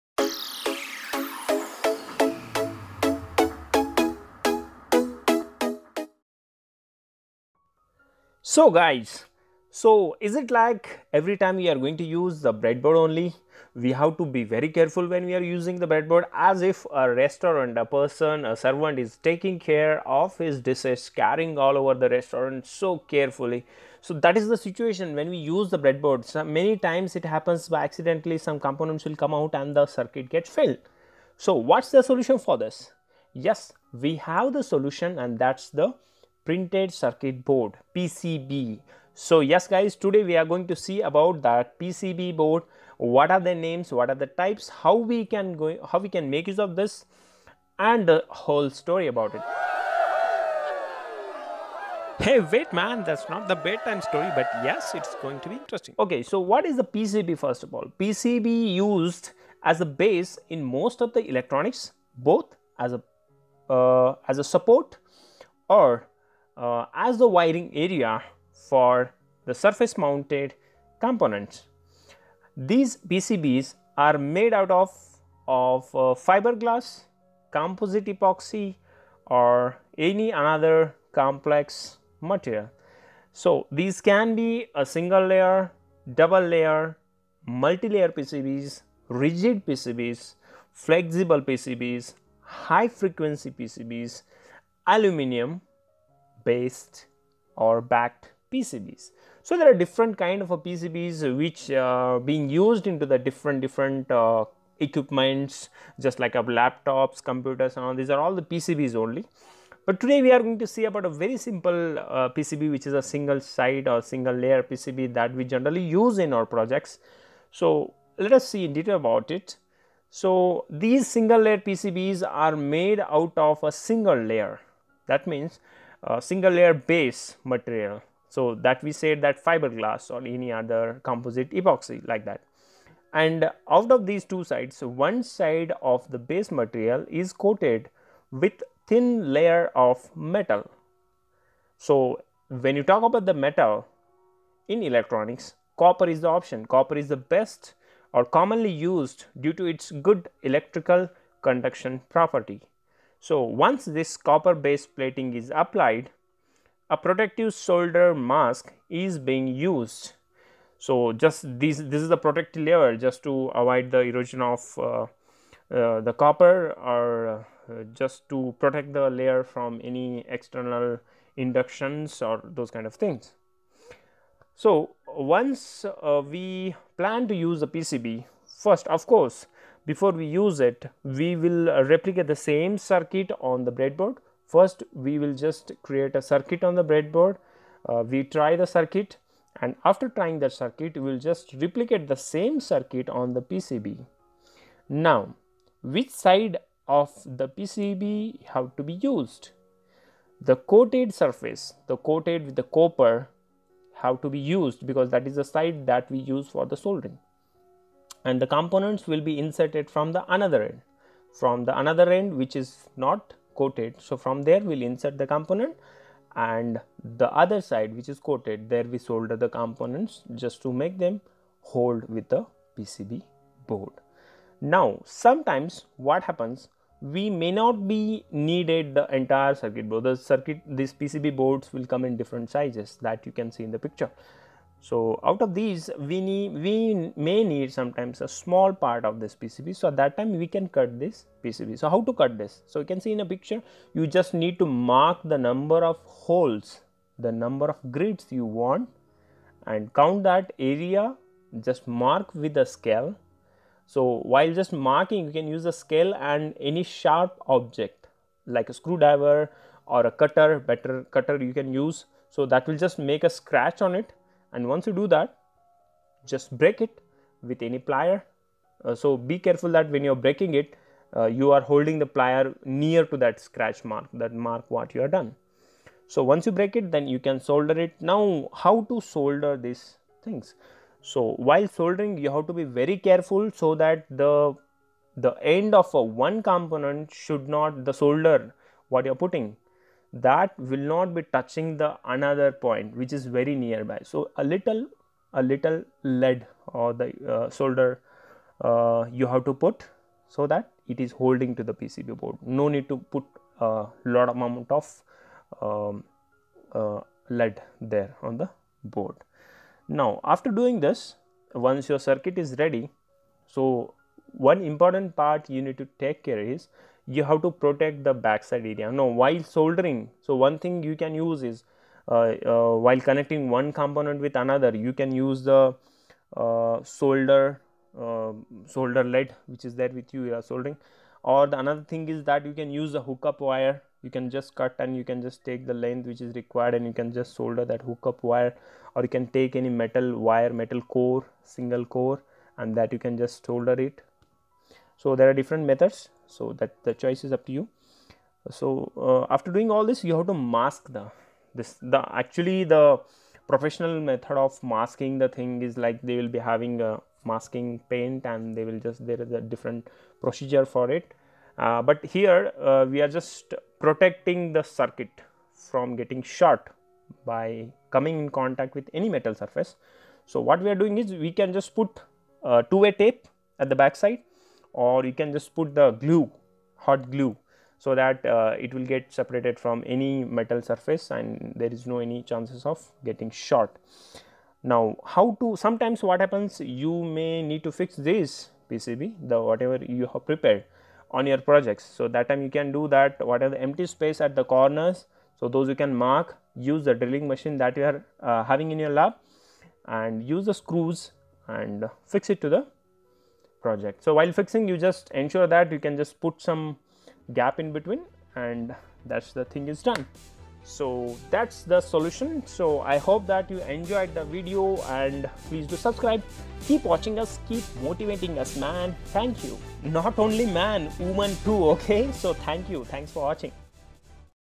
What is Printed Circuit Board | Types | Tricks | PCB Soldering | PCB Cutting | ZERO PCB in detail
HI dear viewer

 A lot of things that you can learn here in this video tutorial. The types of PCB that we generally see in our day to day life. Also specifically you will be able understand the usage of zero PCB (single layer PCB). Also how to mark and cut the single layer PCB.

Happy tinkering.

Team
ROBOSTEM LEARNING SOLUTIONS